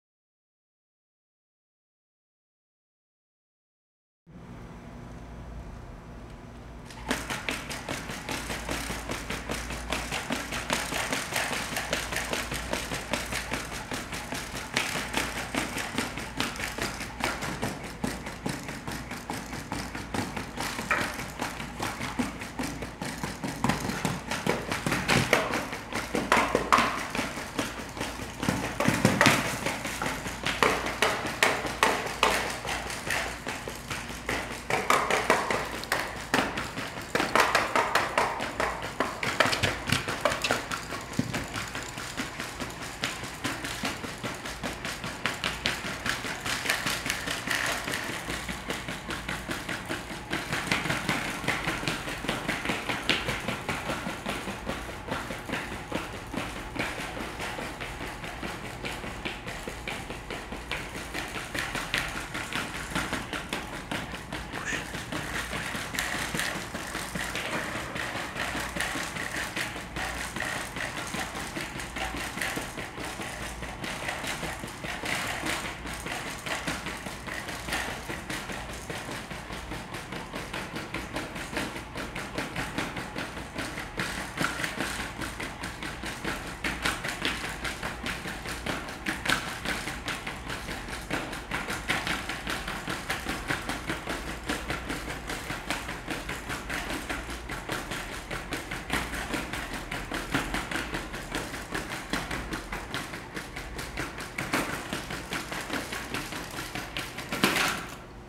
Solo12 Reactive Stepping in New York / NYU
The video shows the current progress of implementing reactive stepping on the Solo12 robot platform at NYU in New York. The work was done in the Machines in Motion Laboratory

The robot in the video is open source.